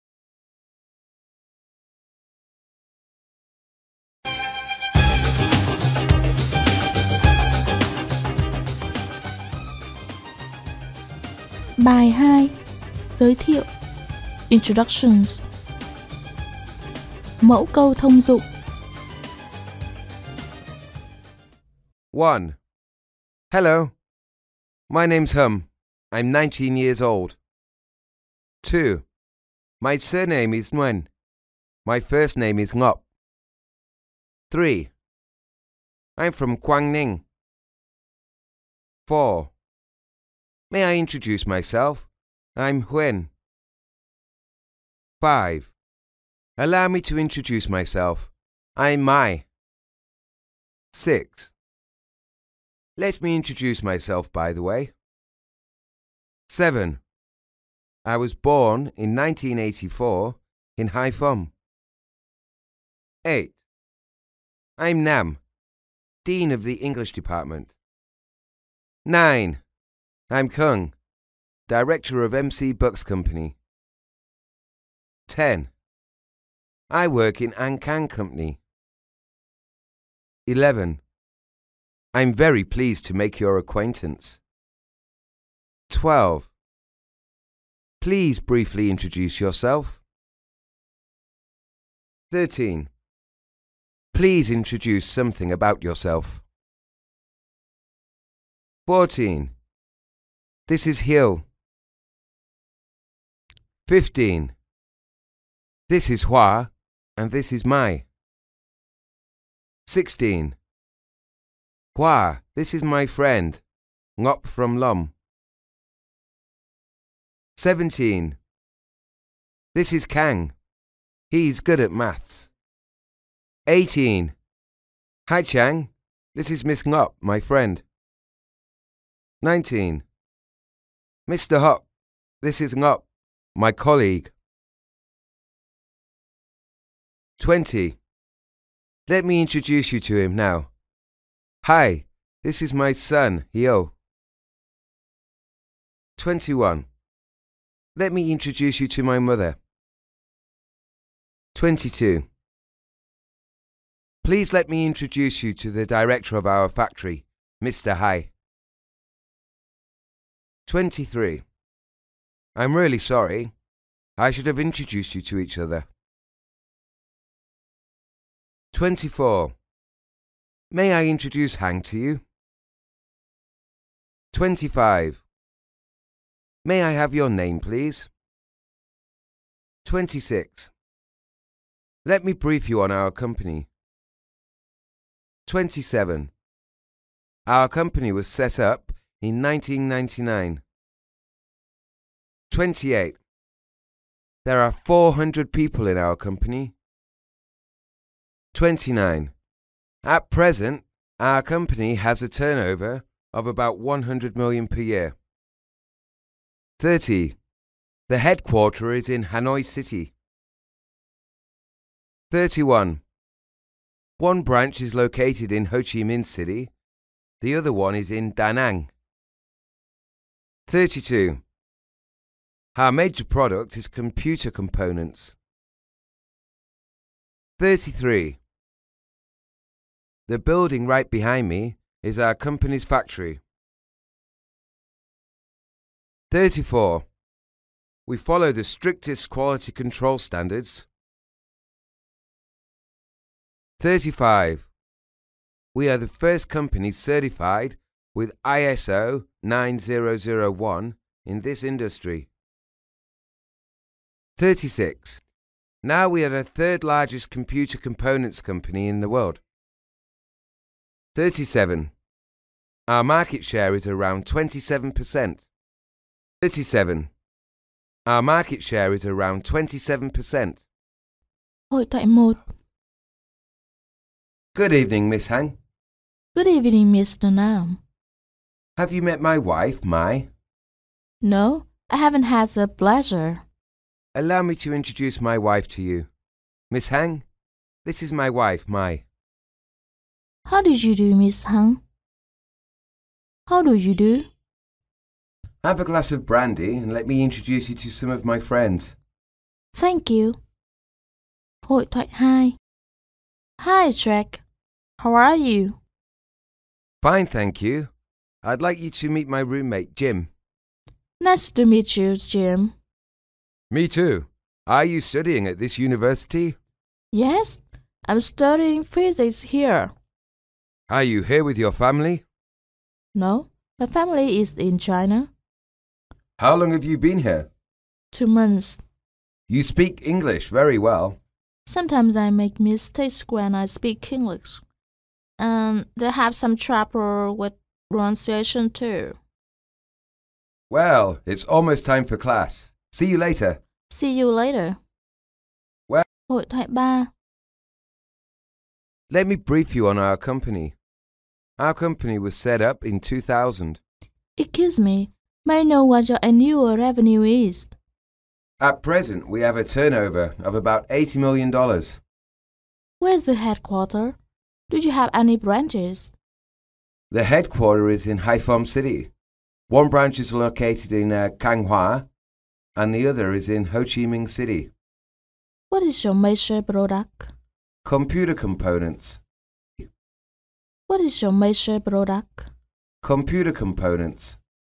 10 phút Tiếng Anh mỗi ngày: Bài số 2
Học Tiếng Anh giao tiếp mỗi ngày. Hôm nay chúng ta lại dành 10 phút để luyện nghe nhé !